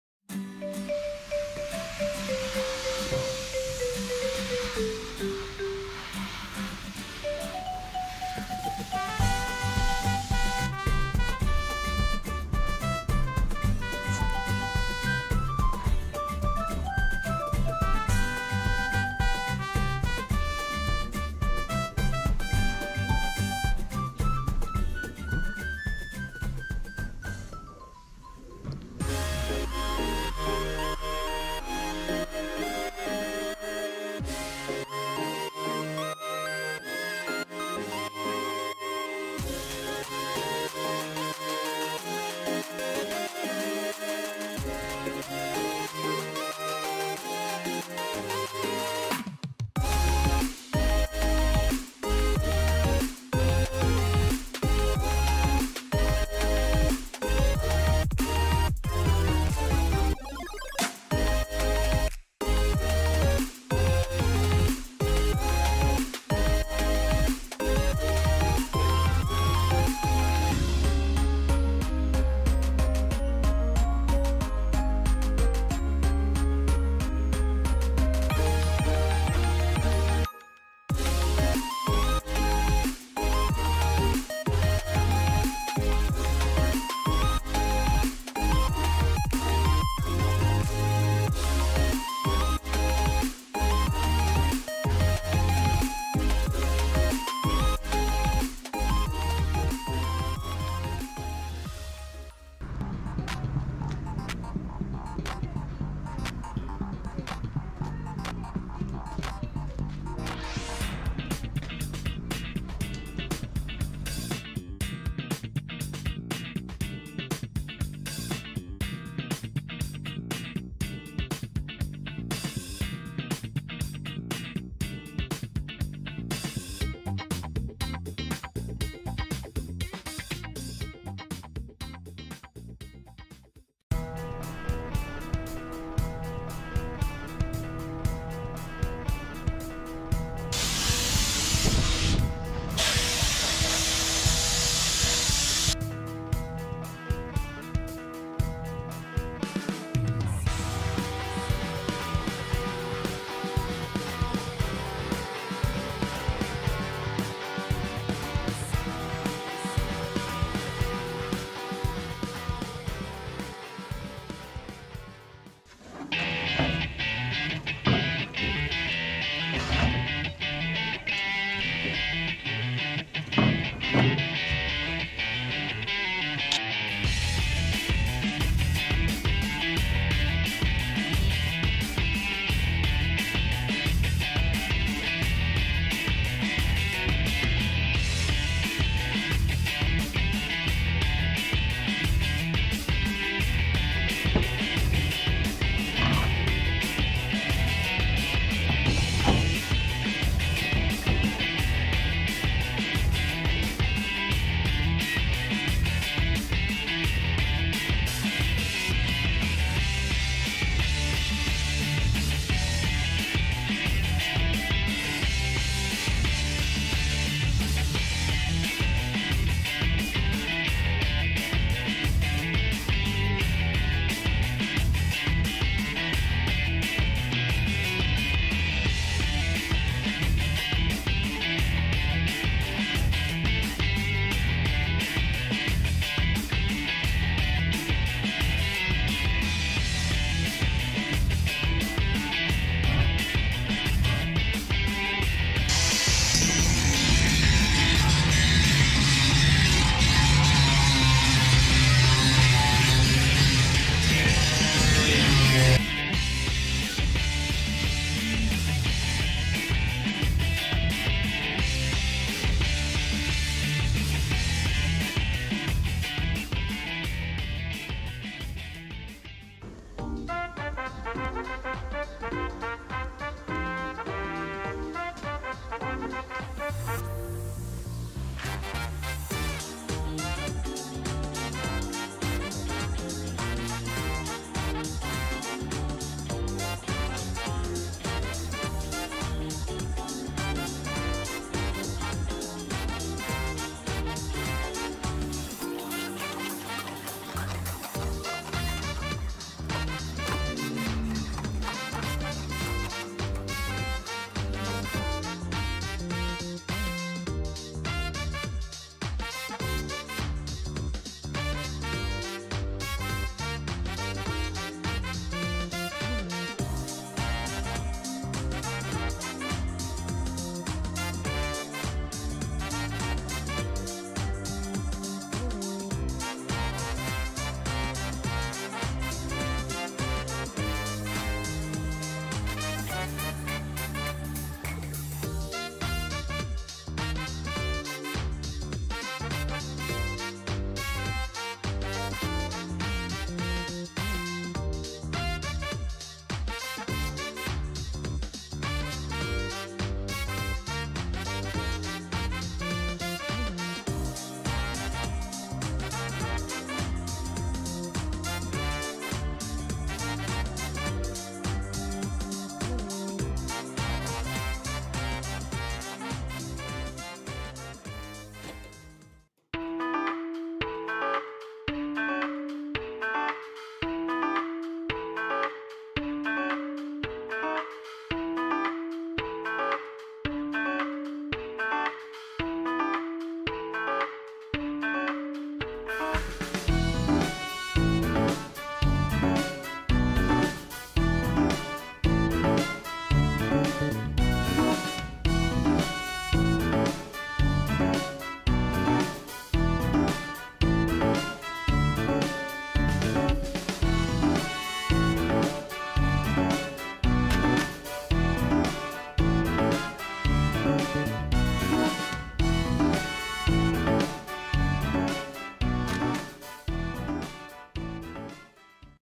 飛鳥の店長日記第81回め♪部品取りAE86をフルレストア！！②
オールカーサービス鴫原社長が撮影です！！
飛鳥ちゃんの体調不良をカバーしていただきました。
ノスタルジックカーTV全体をご覧になる方は
以下のHPより何時でもご笑覧下さい。